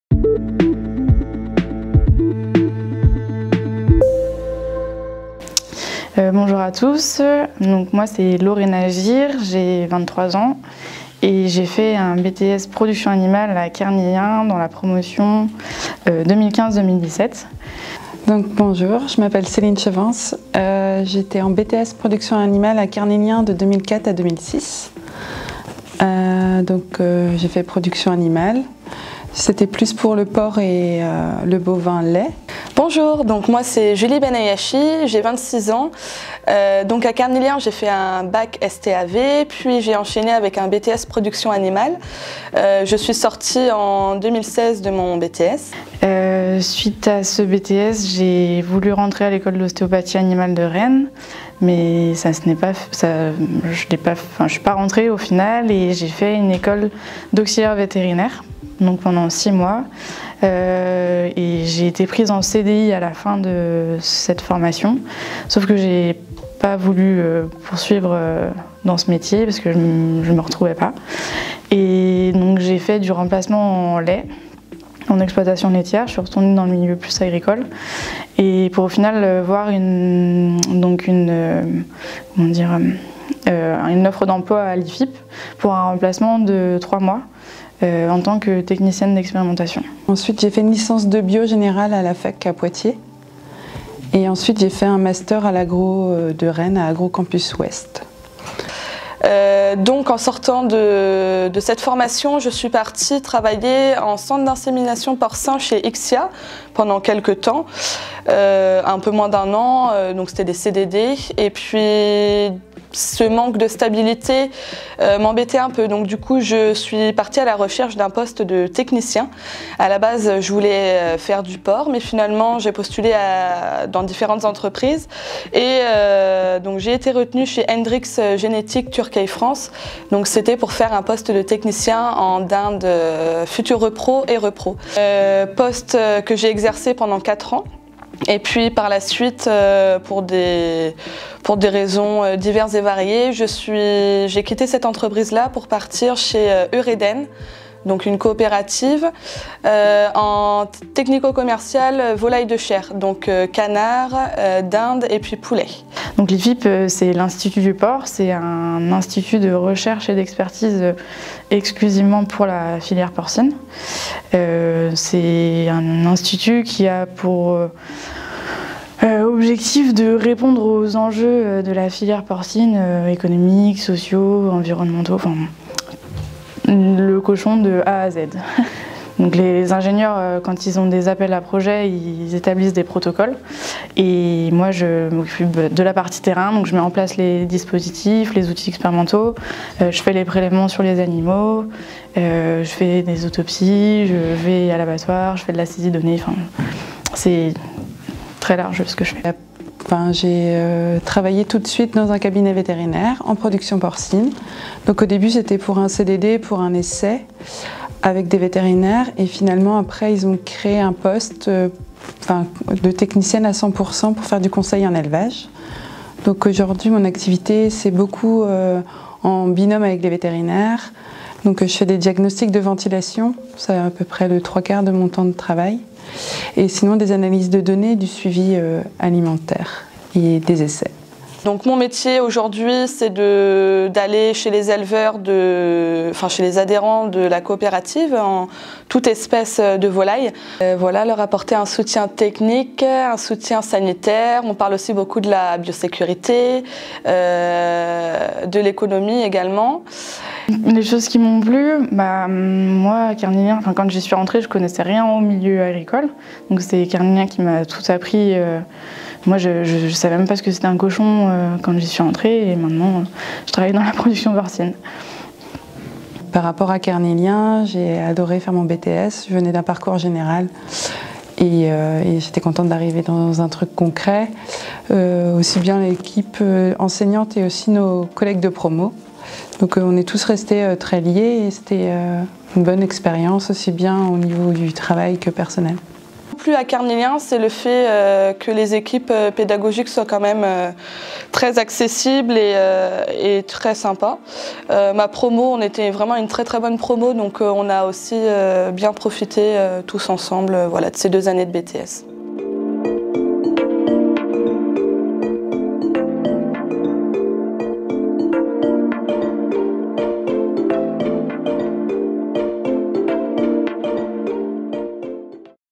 Témoignages d'anciens élèves de BTS à Kernilien : épisode 5
Parcours suite au BTS, emplois et expériences dans les métiers du conseil en élevage : regardez le témoignage de Laurena, Celine et Julie